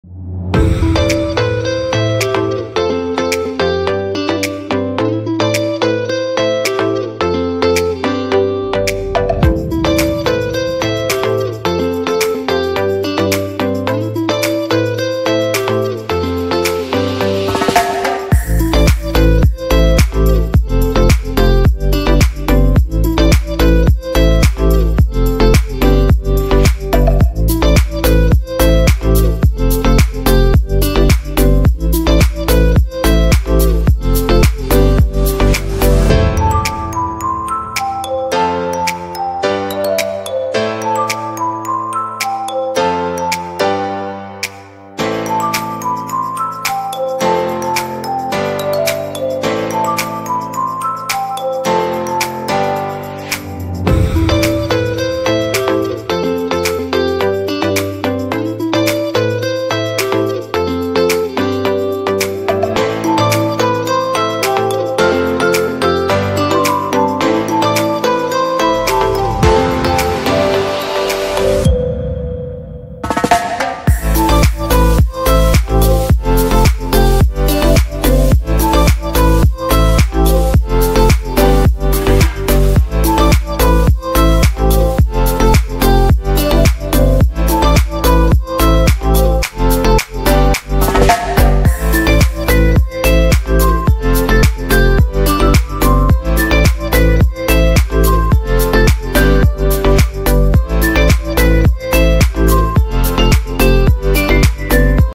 1 dk da diyet çilekli dondurma tarifi
Yazın en çok sevilen tatlısı dondurma.Herkesin sevdiği bir lezzet.Ama bir o kadar da kalorisi yüksek.Özellikle hazır dondurmaların kalorisi çok yüksek. Bu tarif hem çok kolay hem de çok az kalorisi var ve en doğalından. Deneyecek olanlara afiyet şifa olsun.Kanalıma destek olursanız çok sevinirim...
Malzemeler :
Dondurulmuş 15-20 adet çilek
2-3 yemek kaşığı süt
İsterseniz bal da ekleyebilirsiniz
Külah
Şekerleme süslemek için

Müzisyen: @iksonmusic